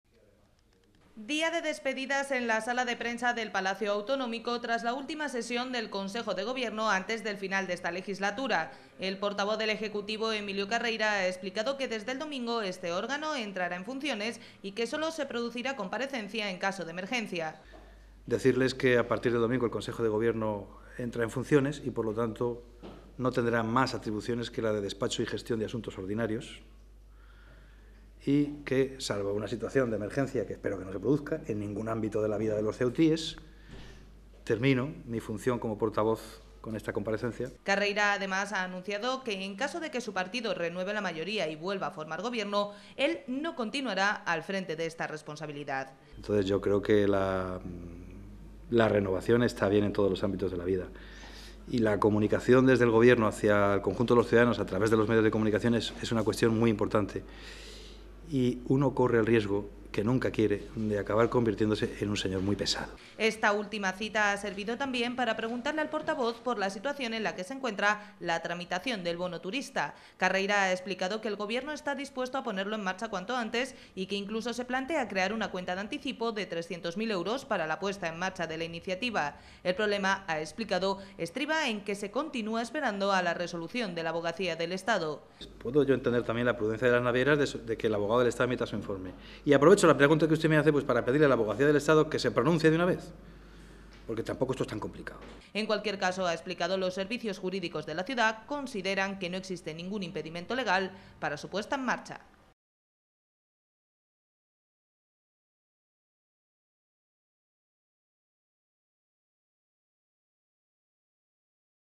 Carreira anuncia que aunque el PP revalide la mayoría no seguirá siendo portavoz del Gobierno
El Consejo de Gobierno ha celebrado este viernes su última reunión de la legislatura antes de su entrada en funciones del próximo domingo, una cita en la que el actual portavoz, Emilio Carreira, ha confirmado que no lo seguirá siendo si el Partido Popular renueva su mayoría.